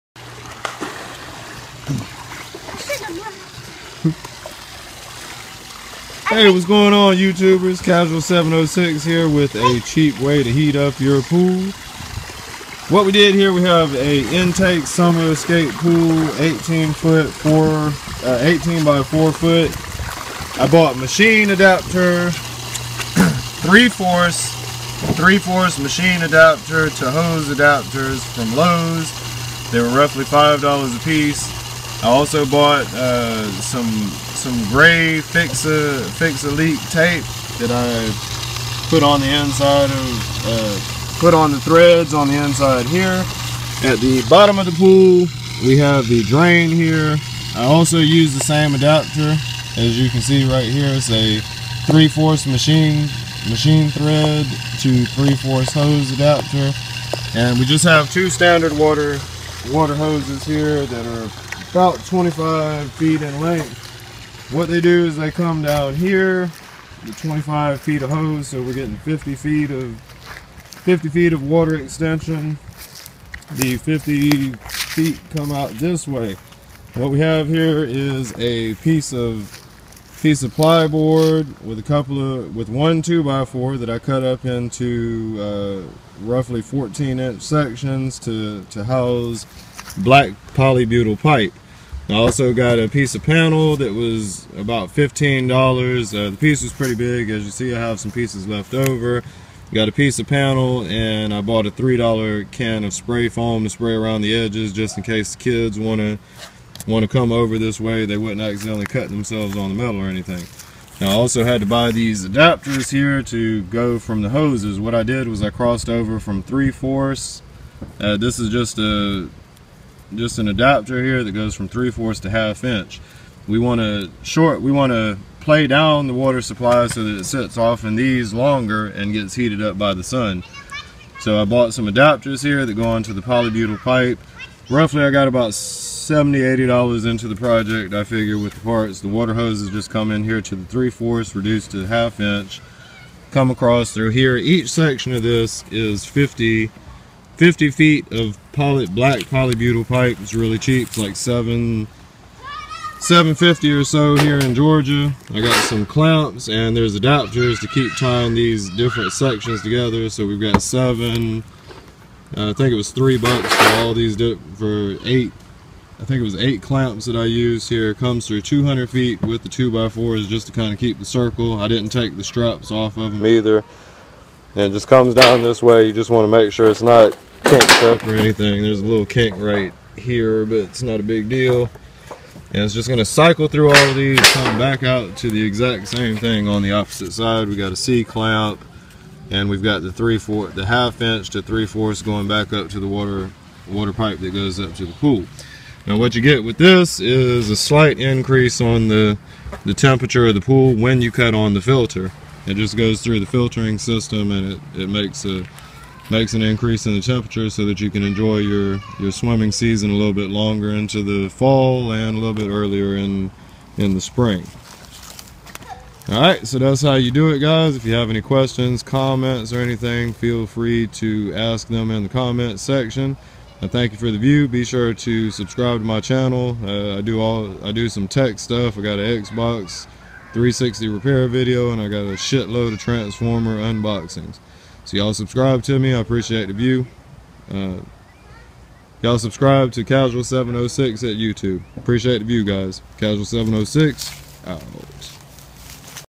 Solar Powered Pool Water Heater For Under 80 Bux!!
A lil different from the normal Tansformers unboxings. Here's a DIY solar powered water heater that I came up with that will gradually increase the temperature of you pool water once the pool filter is cut on. The project cost less than 80 dollars to put together, so I figured I'd share the info.